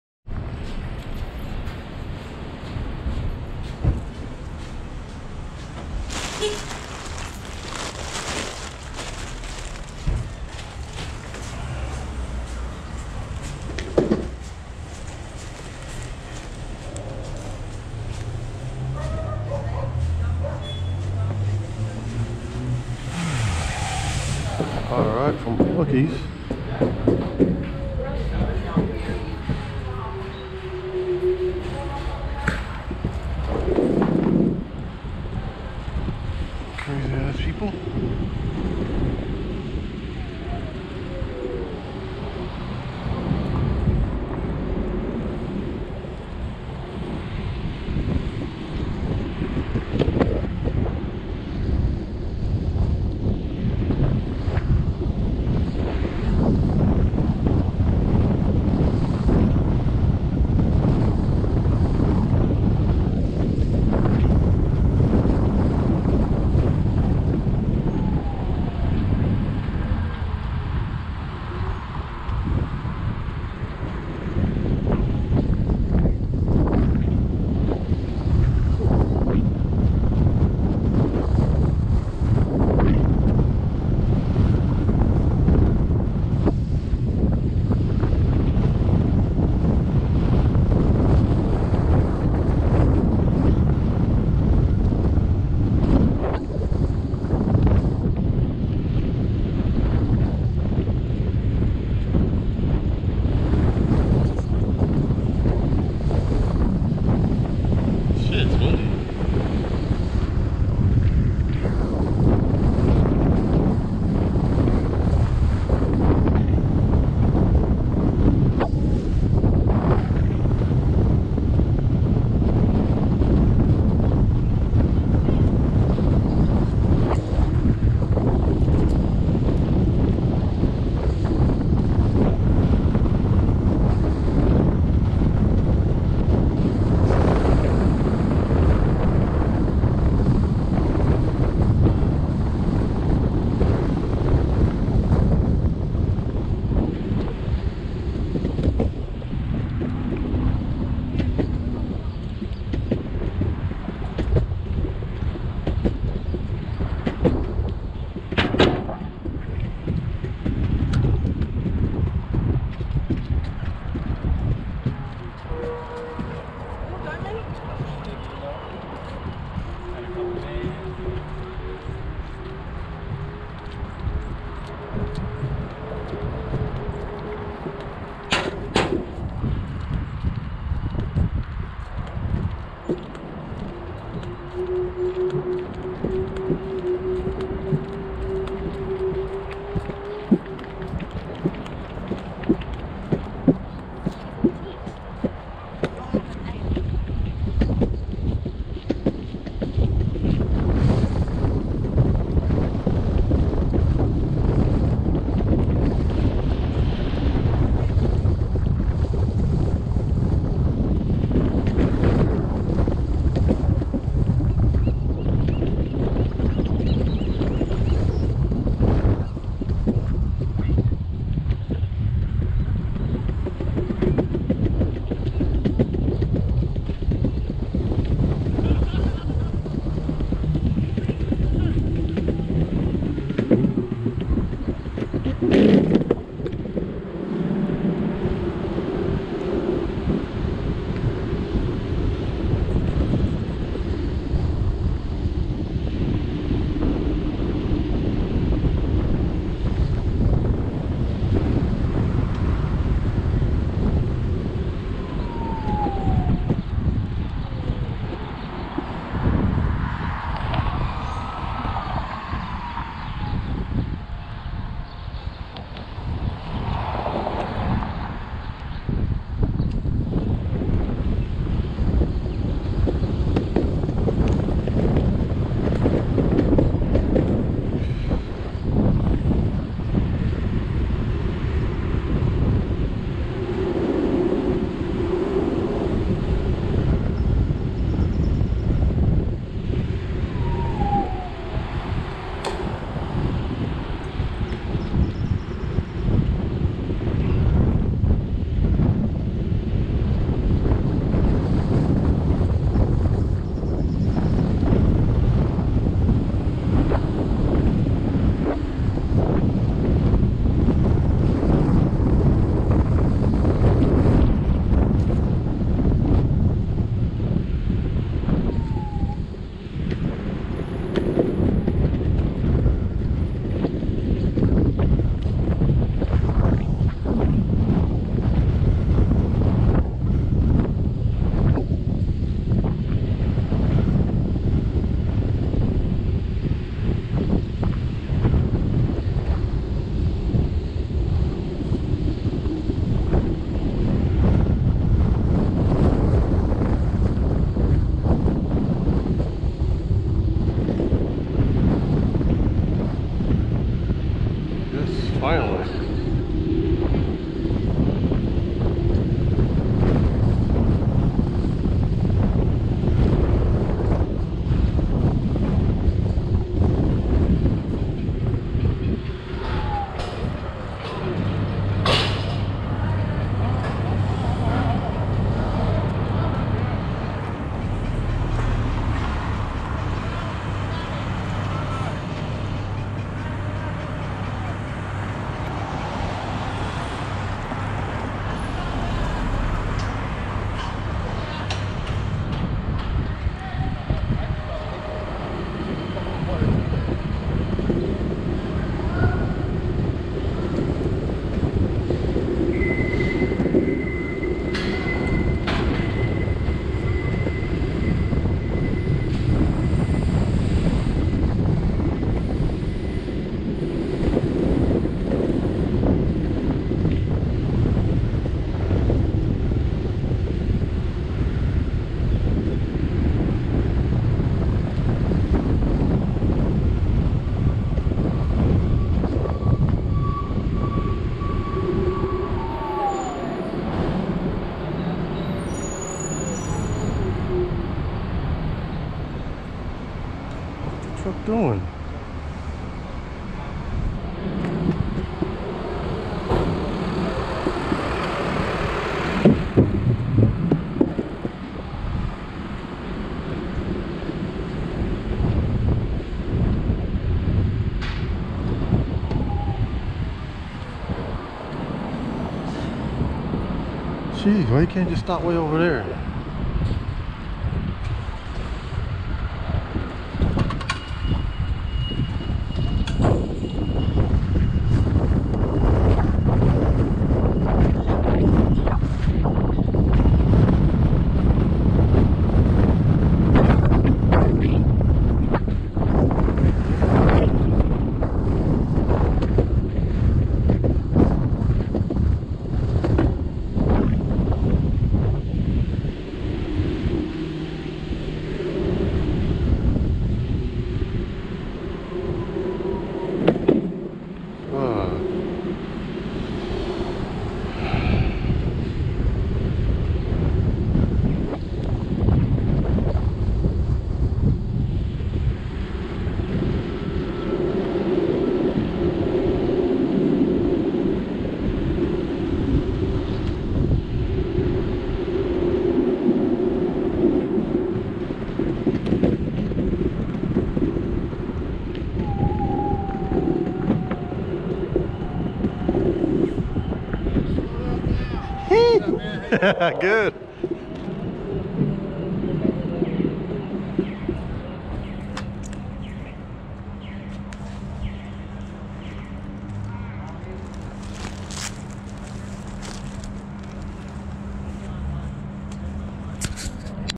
luckyrun shelterinplace
just cruzing around checking out luckys by the lake